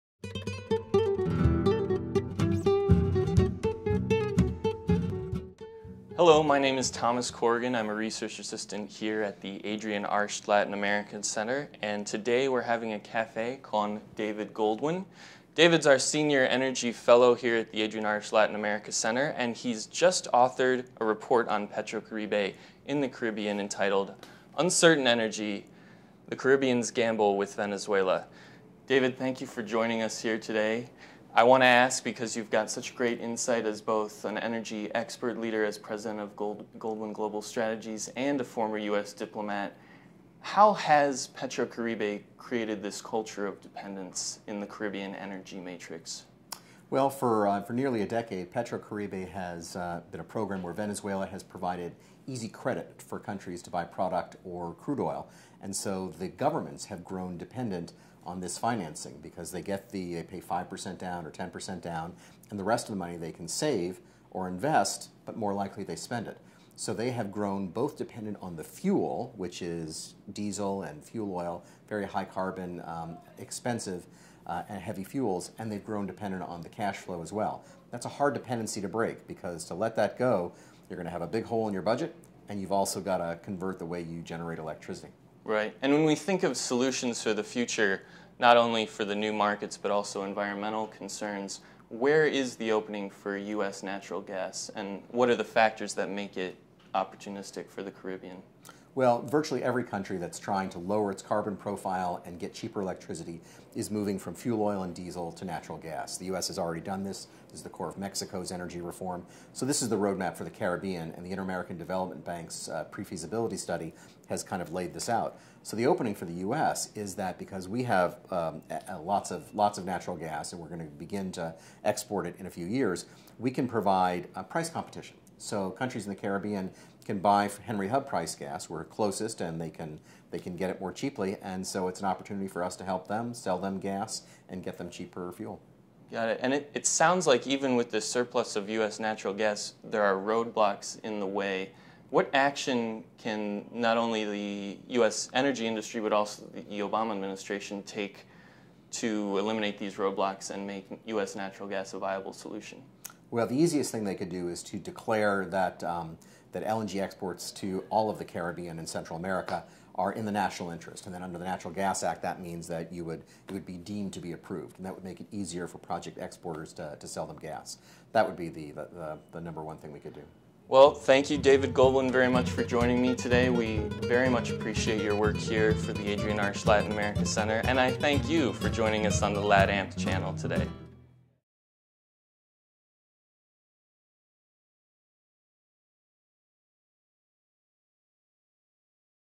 Un Café Con... David Goldwyn
Senior Fellow David Goldwyn sits down with Adrienne Arsht Latin America Center's Thomas Corrigan for a coffee and a conversation on a new report he's authored, "Uncertain Energy: The Caribbean's Gamble with Venezuela"